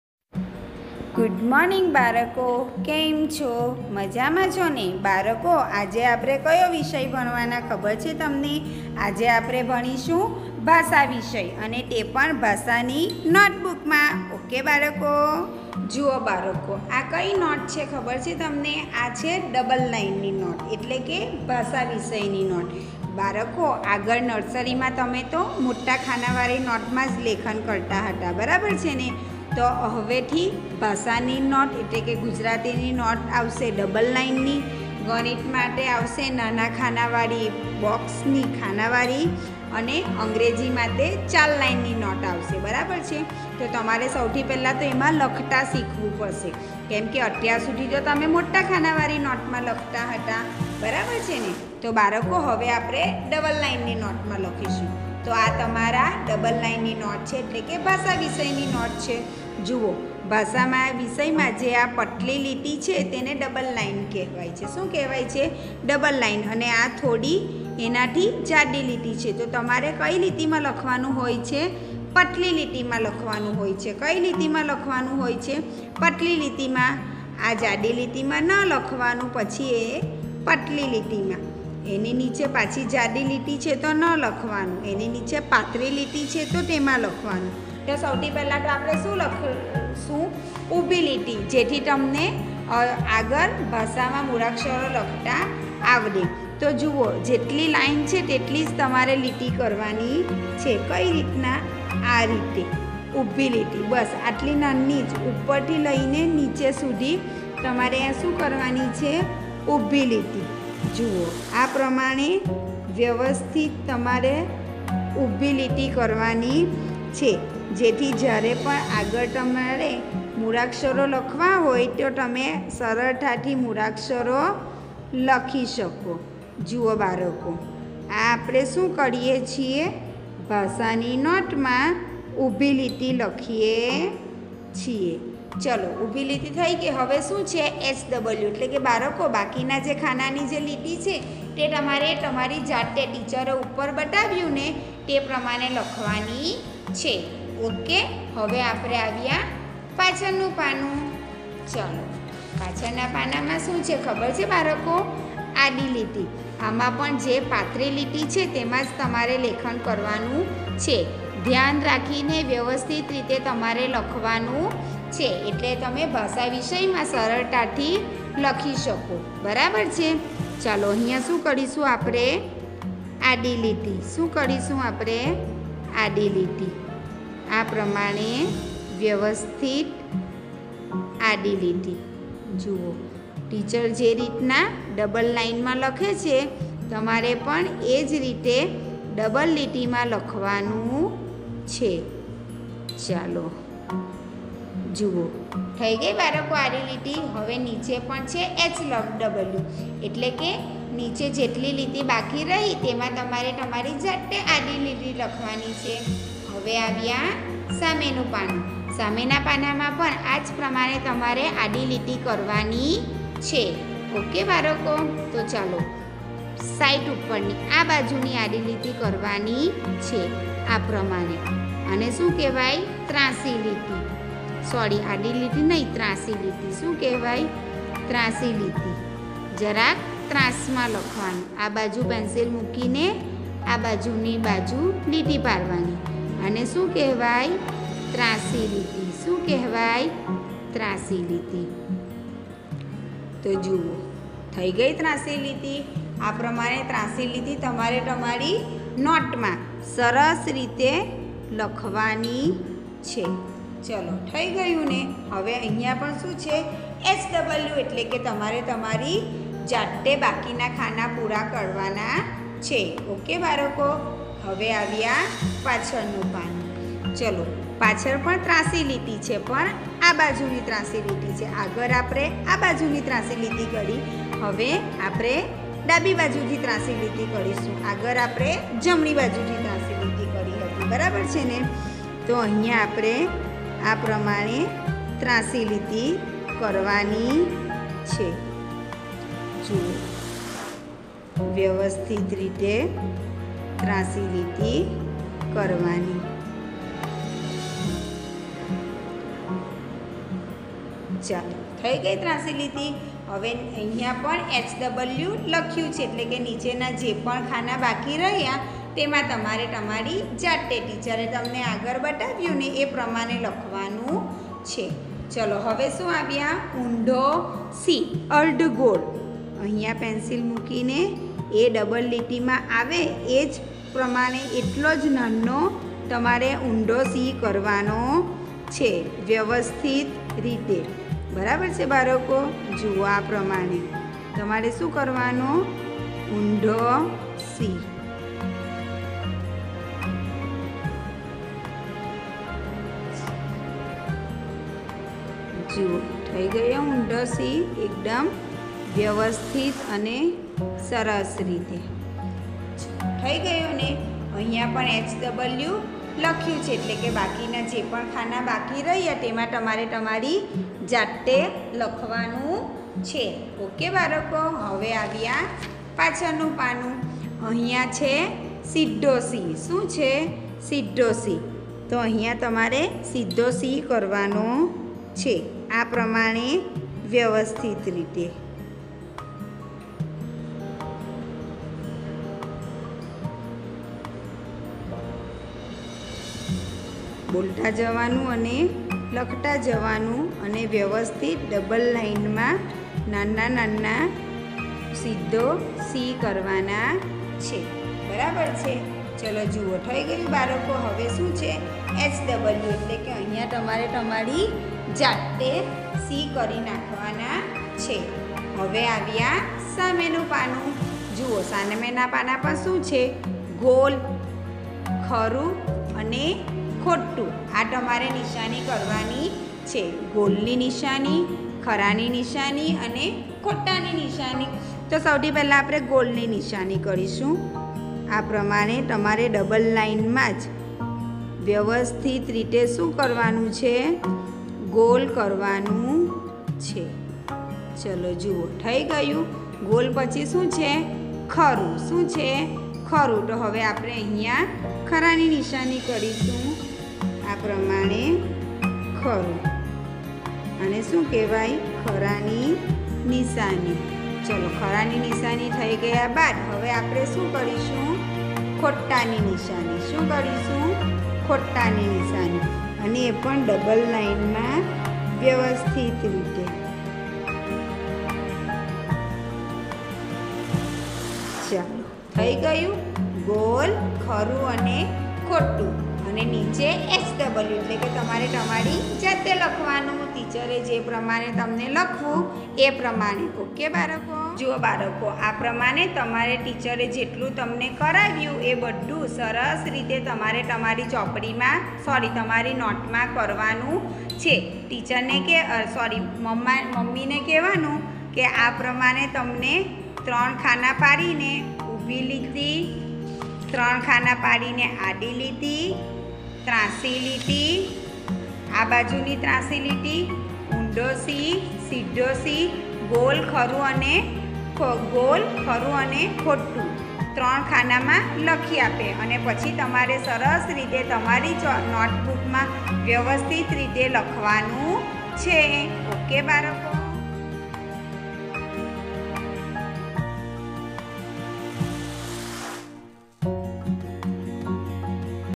ધોરણ-જુ.કે.જી. (ગુ.મા.) વિષય- ભાષા નોટબુક ભાગ - ૧
ઊભી લીટી /આડી લીટી /તાંસી લીટી
/ સીધો સી /ઊધો સી/ અને ગોળ, ખરું, અને ખોટાની નિશાની.(૦૮:૫૫)